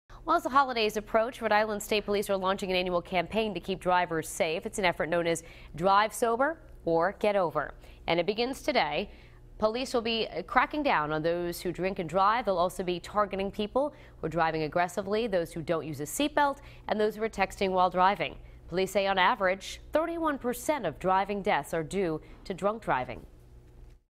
State Police Launch Annual 'Drive Sober' Campaign
As the holidays approach, troopers will be cracking down on those who drink and drive.